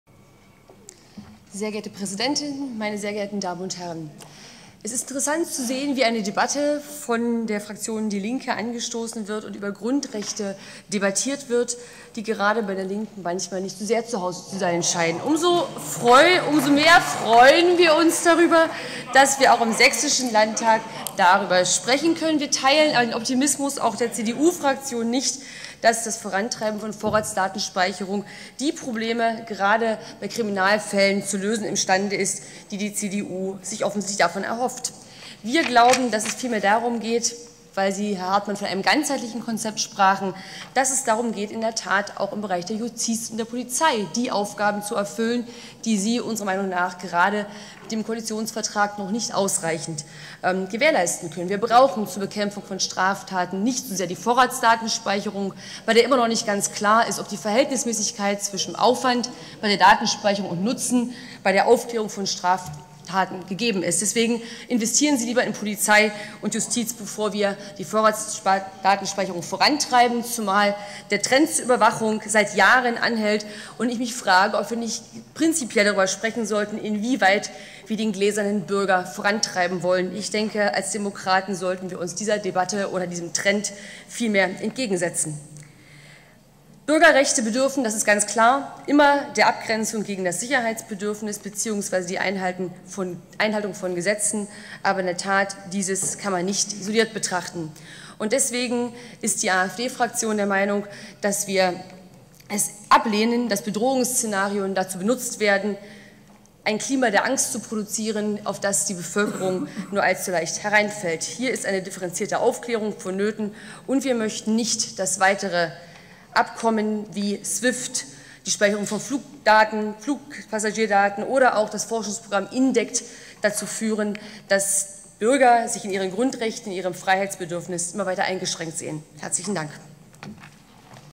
AfD-Fraktionsvorsitzende Dr. Frauke Petry zur Vorratsdatenspeicherung
Aktuelle Debatte im Sächsischen Landtag am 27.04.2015 zum Antrag der Linken